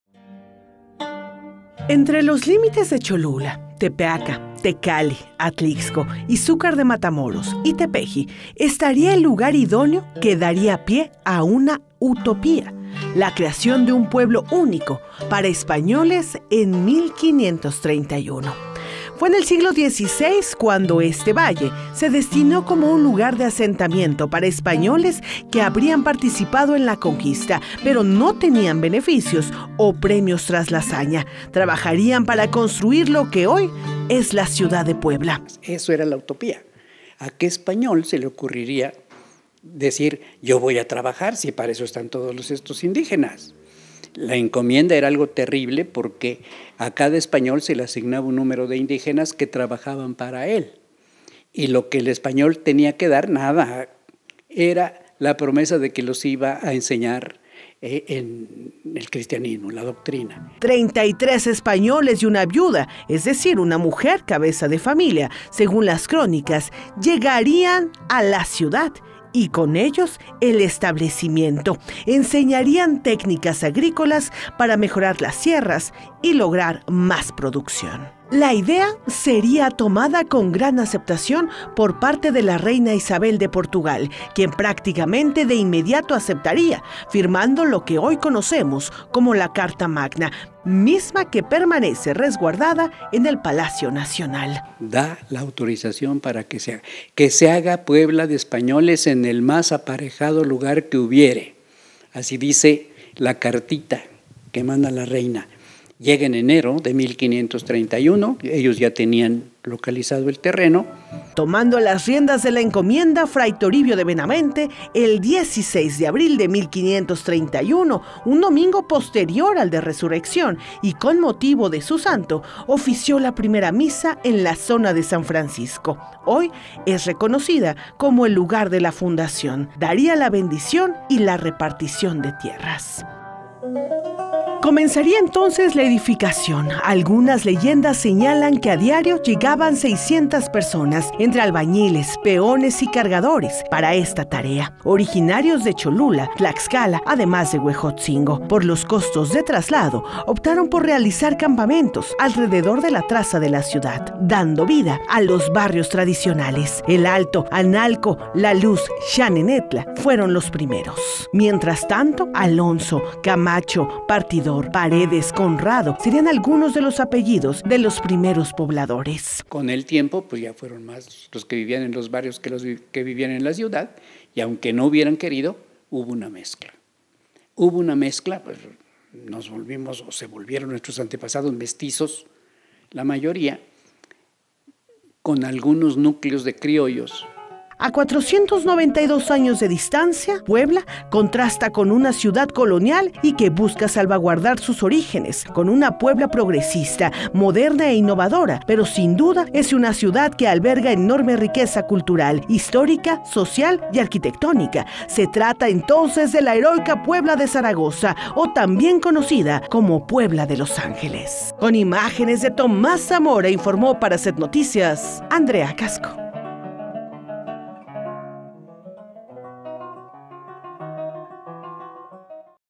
Se cumplen 492 años de de la fundación de la capital poblana.
📰 setnoticias.mx